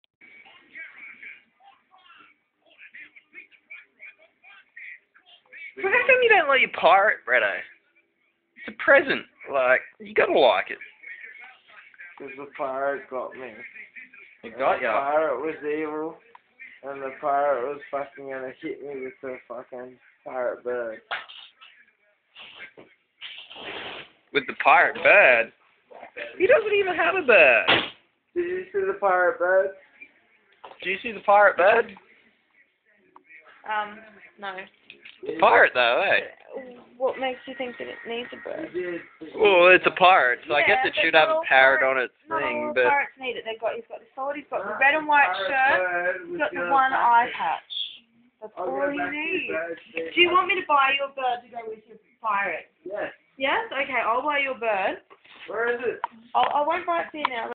Drunken Brett with no pirate
Brett having a drunken cry cos his pirate has no parrot. :(